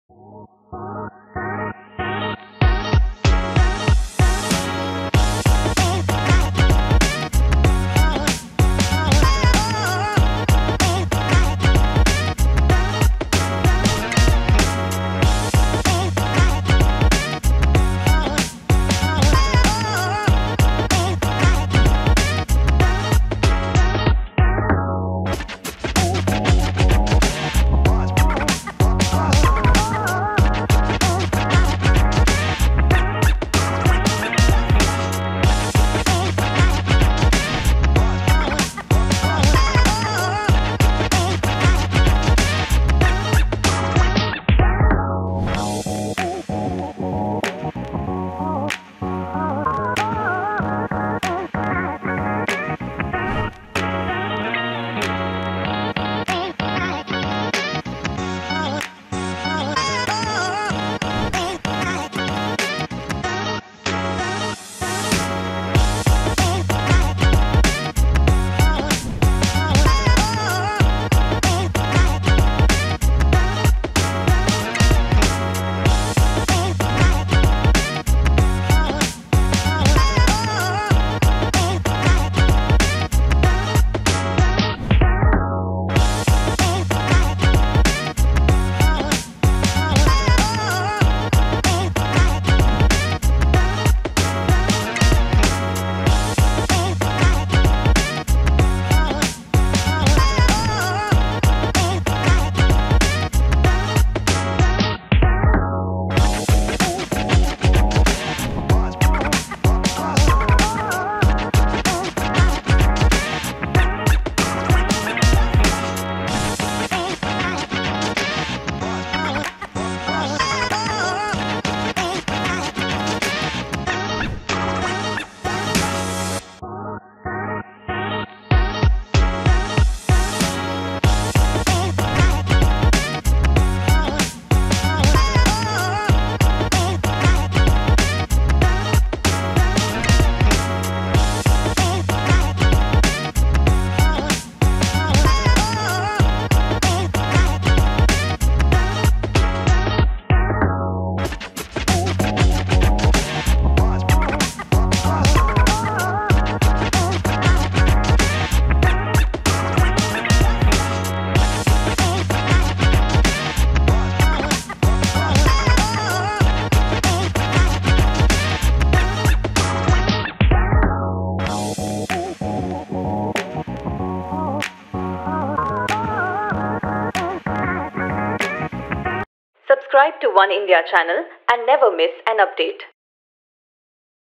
Tejasswi Prakash celebrates her 30th birthday | Lesser know facts about the actor | Oneindia News
Actor Tejasswi Prakash celebrated her 30th birthday with her boyfriend Karan Kundrra and her family. For the celebration, Tejasswi donned a gorgeous red gown.

Know some lesser know facts about the actress.